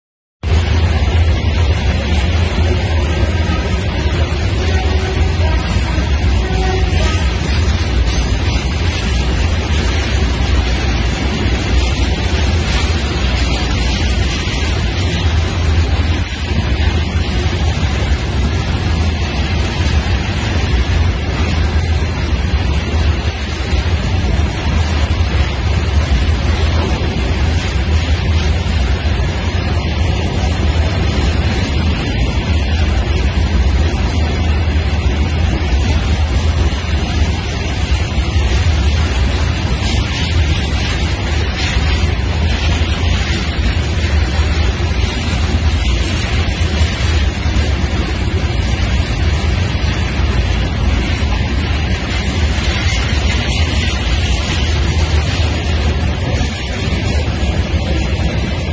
KİRPİ zırhlı araçları Romanya’dan geçerek Ukrayna’ya gitti
*Ukrayna ordusu için tedarik edilen Türk yapımı KİRPİ 4x4 zırhlı araçları, tren ile Ukrayna'ya sevk ediliyor.

*Gazete Balkan Romanya'da ve Balkanlar'da Türklerin sesi olmaya devam ediyor.